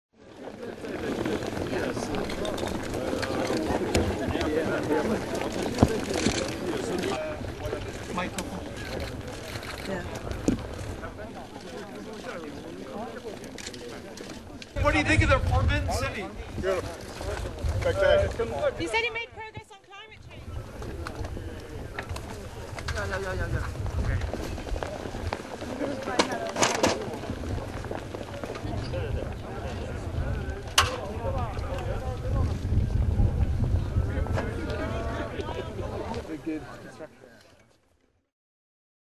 Obama tours Forbidden City
US President Barack Obama took time from a busy schedule during his first, high-profile visit to China to tour the country's Forbidden City in Beijing on Tuesday.